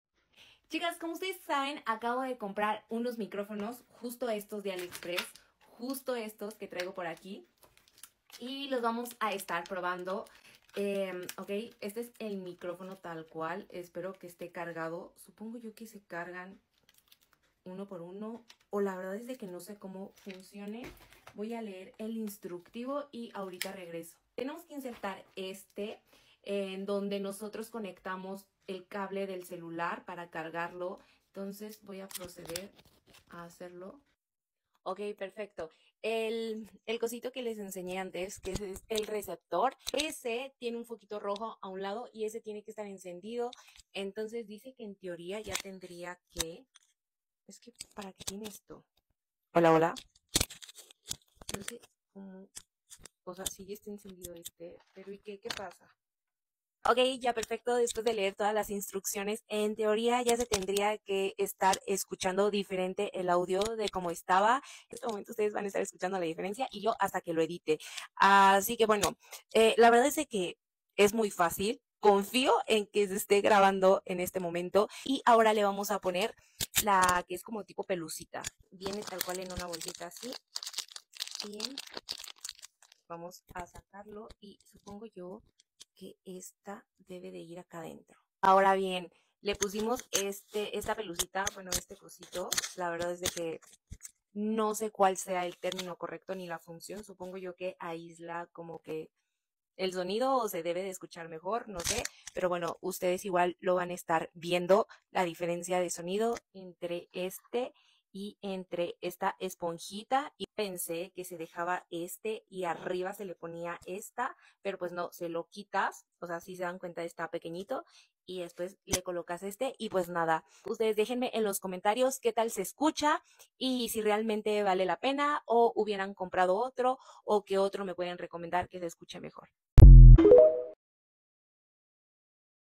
Micrófono para Celular, están padrísimos 🎤🛍️❤️
Chicas los probé y se ven de muy buena calidad y el sonido es claro, los estaré probando todo un mes y les platicaré cómo me fue  ❤️💕

Les mando un abrazo ❤️❤️